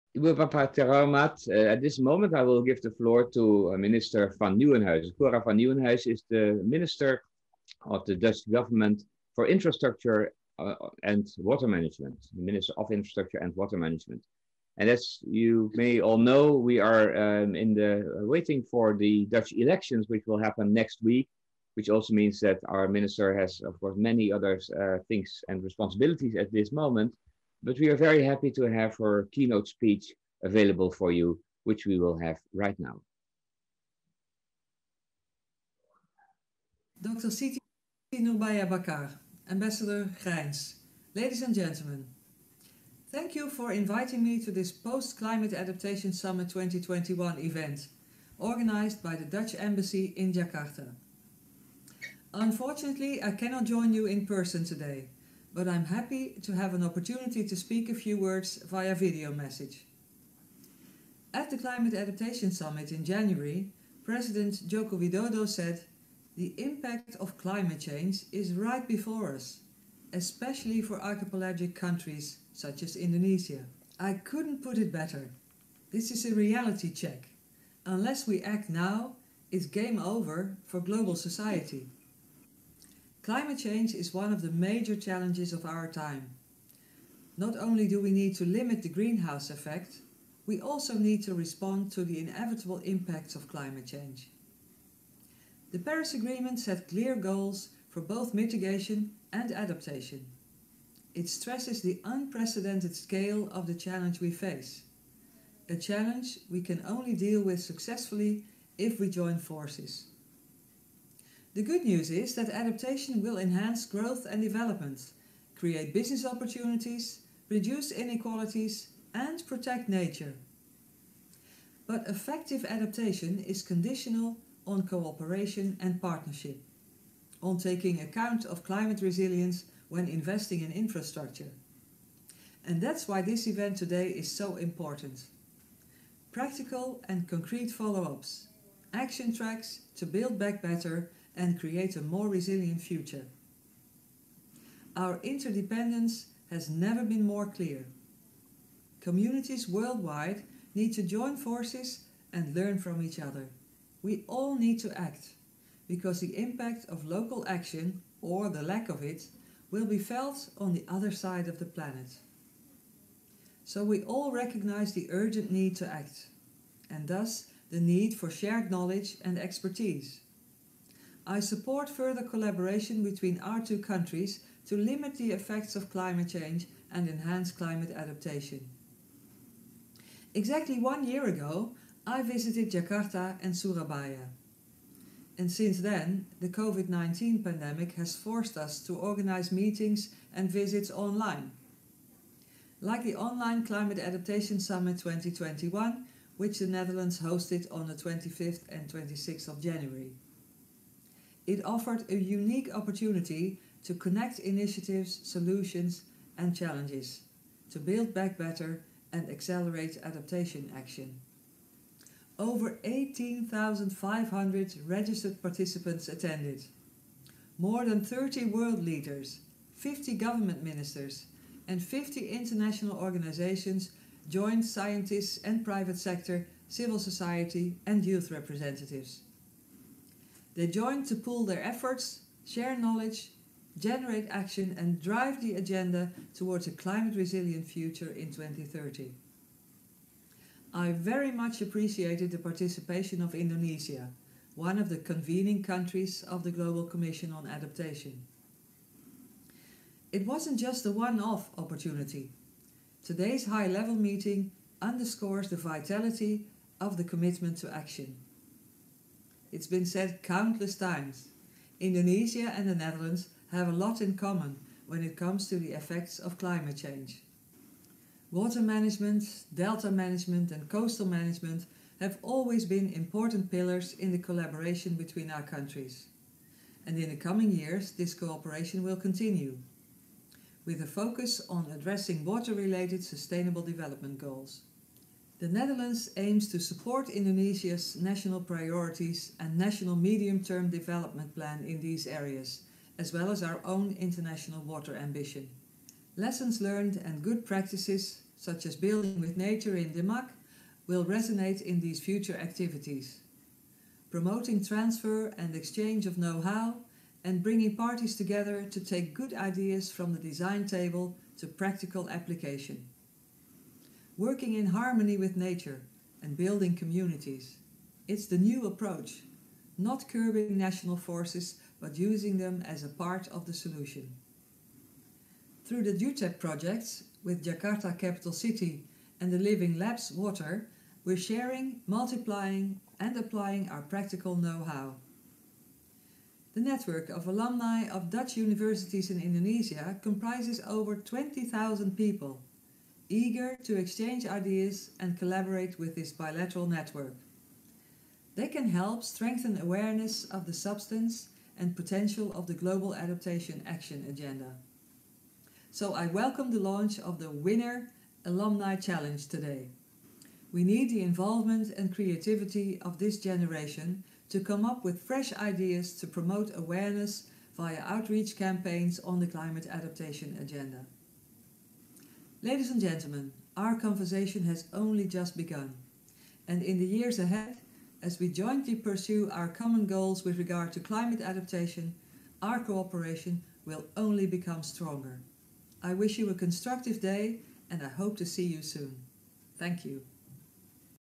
WINNER follow up to the CAS2021 - Keynote Speech : Mrs. Cora van Nieuwenhuizen
"Keynote Speech : H.E. Mrs. Cora van Nieuwenhuizen, Netherlands Minister of Infrastructure and Water Management (IenW)"

Delivering the Adaptation Action Agenda in Indonesia
Climate change affects many aspects of the natural and social environment, and can lead to knock-on impacts between different sectors and groups. We have to simultaneously design and implement policies to prevent further climate change as well as adapt to the impact of changes already happening.

This WINNER event is a follow-up of the Climate Adaptation Summit (CAS), January 2021, and explores forms of cooperation between and within nations to make climate adaptation work for everyone, with a focus on Indonesia and the Netherlands. We will build on the successful international establishment of the Adaptation Action Agenda with high level keynotes. Then, two forms of Indonesia – Netherlands collaboration in urban and rural areas will be highlighted. Finally, we dive into the prerequisites for successful climate adaptation in a session focused on a priority for both the Netherlands and Indonesia: water resilience.

During the event, the Indonesian Minister of Environment and Forestry and the Netherlands Envoy for Climate Change, will launch an Indonesia wide NL-alumni challenge, aimed at further strengthening the awareness about the Adaptation Action Agenda as a journey of hope.

Objective: to develop a perspective of hope for climate adaption
To foster Indonesian – Netherlands science and education collaboration in support of the Adaptation Action Agenda
To strengthen dialogue between the knowledge sector, policy makers, businesses and communities in both countries
To strengthen awareness on the global need for climate adaptation and climate neutral policy and initiatives
To highlight existing cooperation initiatives between the Netherlands and Indonesia in this field
To engage the private sector and Indonesian NL-Alumni in this process